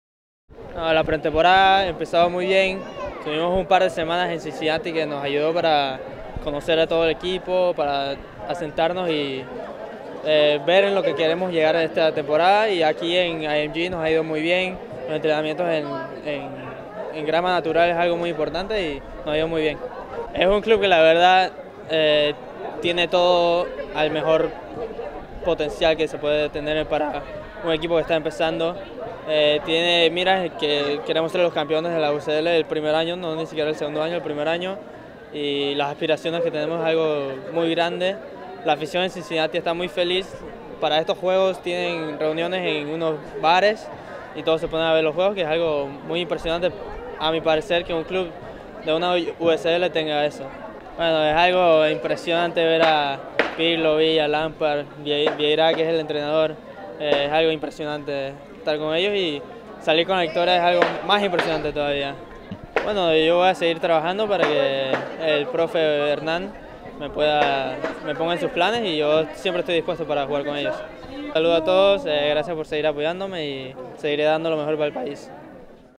Futbol USL- Entrevista con Francisco Narbon
Charlamos con Francisco Narbon, jugador panameño del FC Cincinnati,  después del partido de pre-temporada contra el NYC FC.